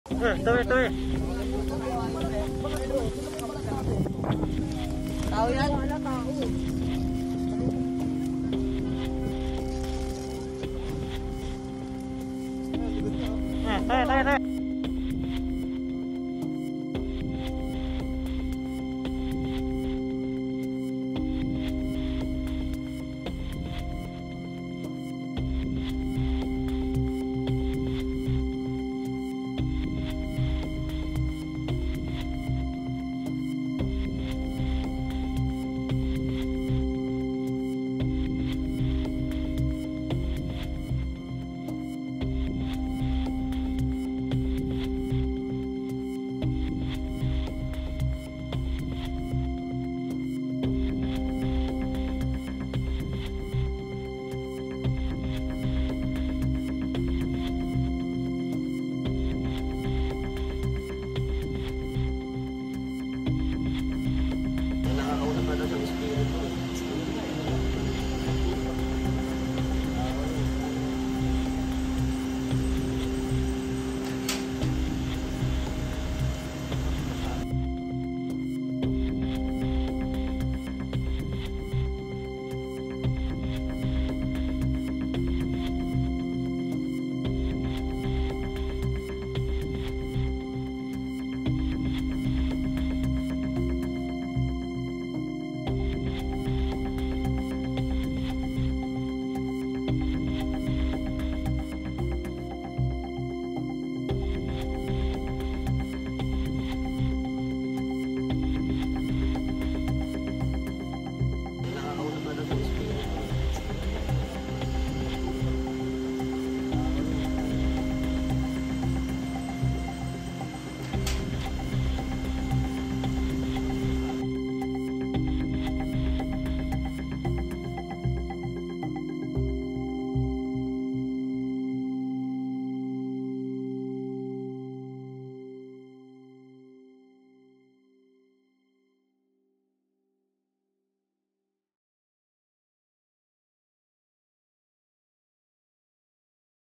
Chop-chop victim, natagpuan sa Bacoor, Cavite | NXT
Natagpuang putol-putol sa Bacoor, Cavite ang katawan ng retired engineer na isang buwan na umanong nawawala ayon sa kaniyang pamilya.